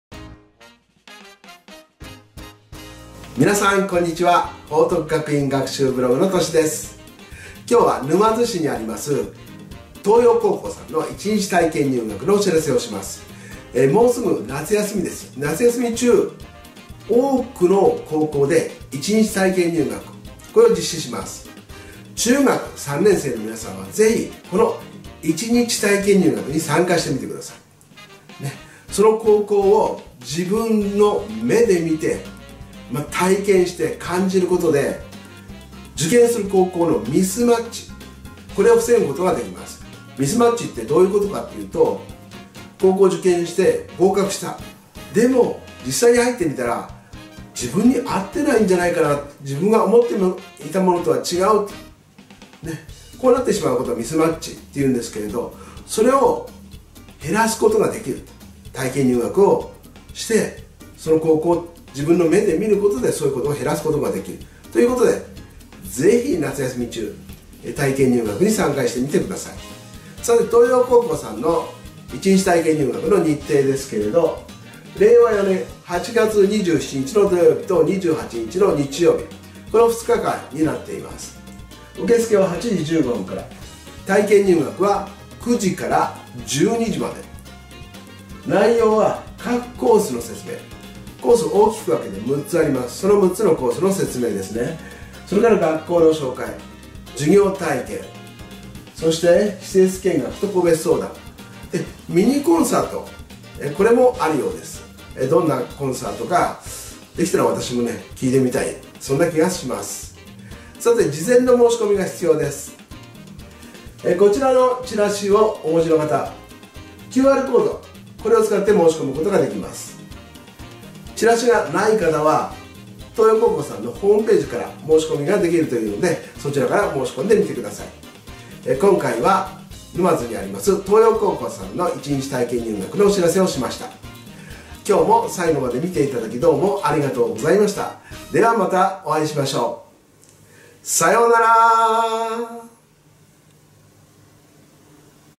桐陽高校一日体験入学 静岡県の学校情報
沼津市にある桐陽高校さんの一日体験入学のお知らせです。